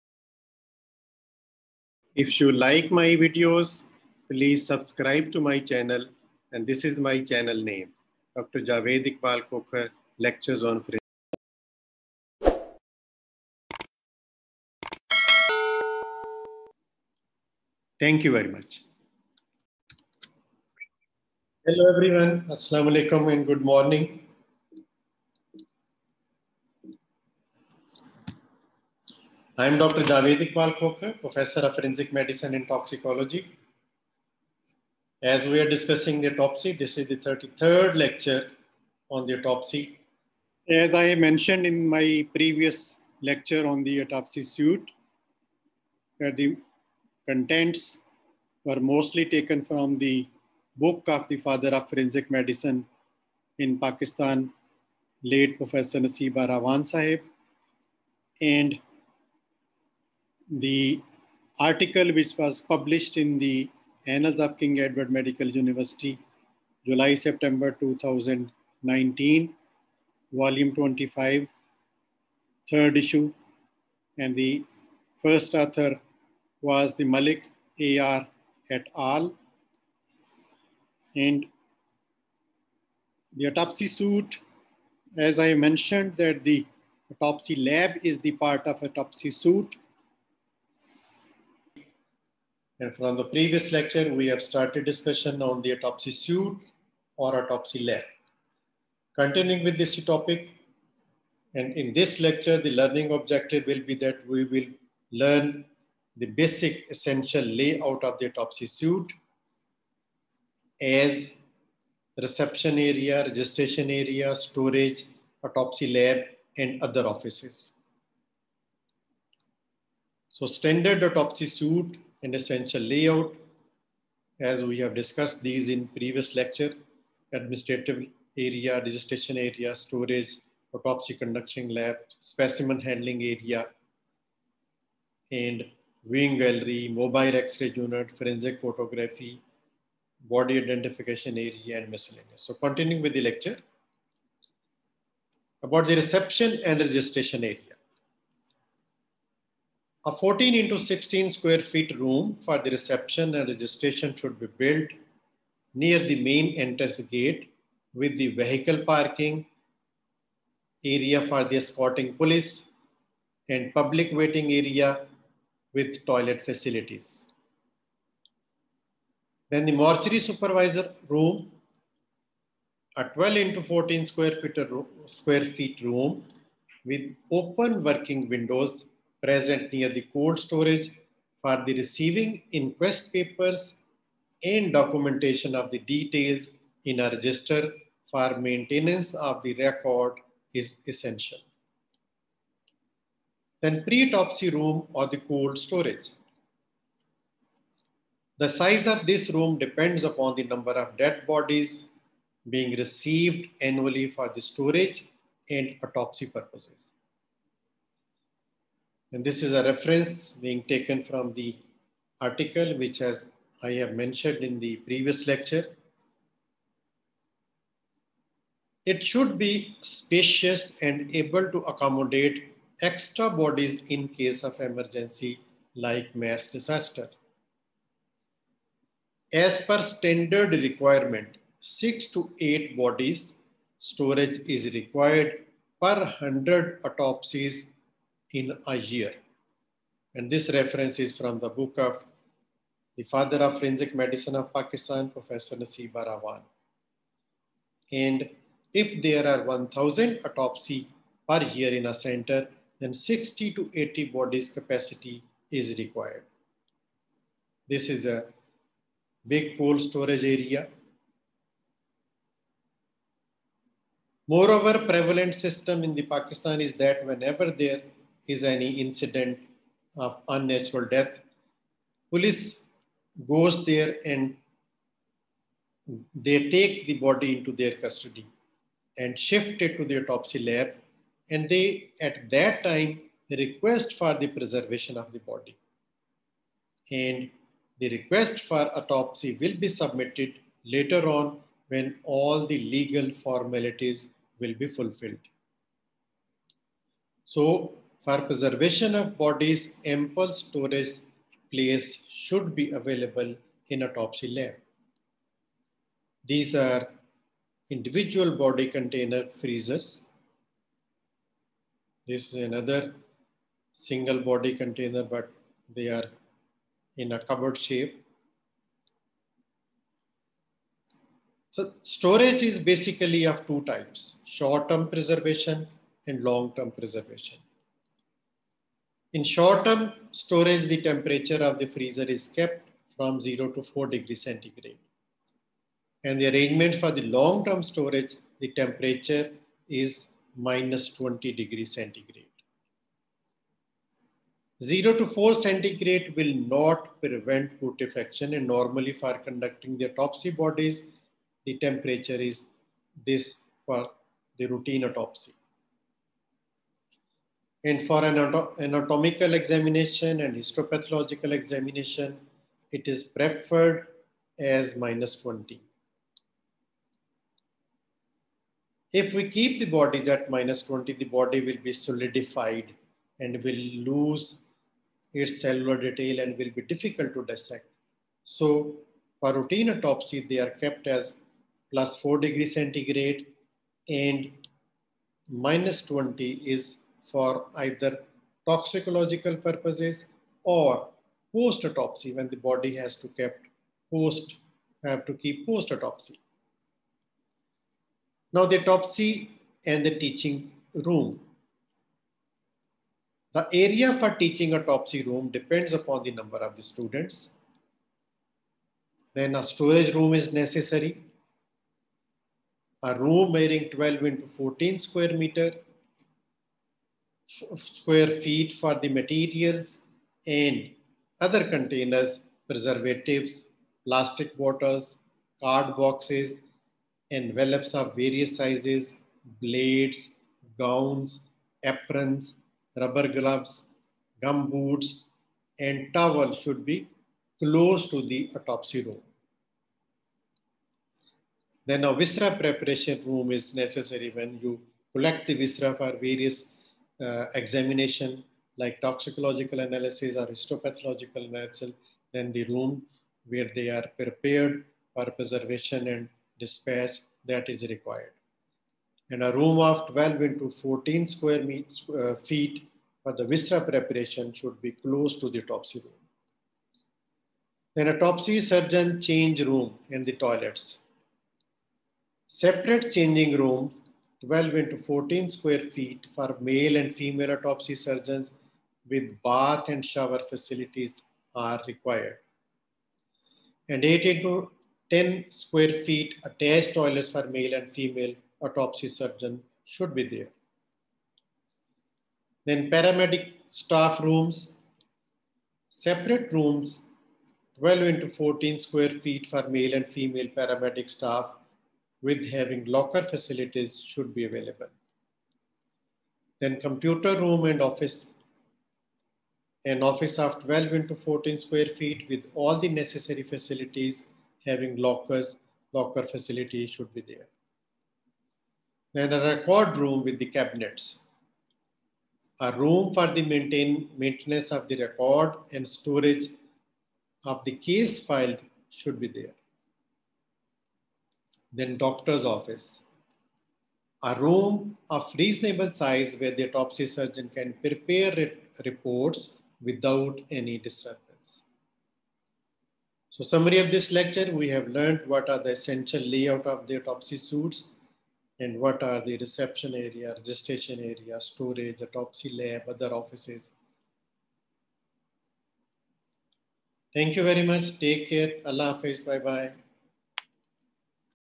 33. AUTOPSY SUITE-2 ESSENTIAL DESIGN OF SUITE AUTOPSY LAB-THE INTEGRAL PART OF SUITE
THIS IS 33RD LECTURE ON AUTOPSY
IN THIS LECTURE I HAVE DISCUSSED ABOUT THE AUTOPSY SUITE.

THE BASIC LAYOUT AND ESSENTIAL REQUIREMENTS ARE DISCUSSED IN THIS LECTURE.
AUTOPSY LAB IS INTEGRAL PART OF THE SUITE OF AUTOPSY.

STANDARD ESSENTIAL DESIGN OF THE AUTOPSY IS FOLLOWING,
WHICH INCLUDES,
1. ADMINISTRATION AREA
2. REGISTRATION AREA
3. STORAGE AREA
4. AUTOPSY LAB OR THE AUTOPSY CONDUCTION AREA
5. DEMONSTRATION/VIEWING GALLERY
6. SPECIMEN HANDLING AREA
7. MOBILE X-RAY UNIT
8. BODY IDENTIFICATION AREA

GENERAL PRINCIPLES ARE SAME FOR ALL AUTOPSY SUITES WITH MONOR DIFFERENCES, DEPENDING UPON THE LEVEL OF HEALTH CARE FACILITY.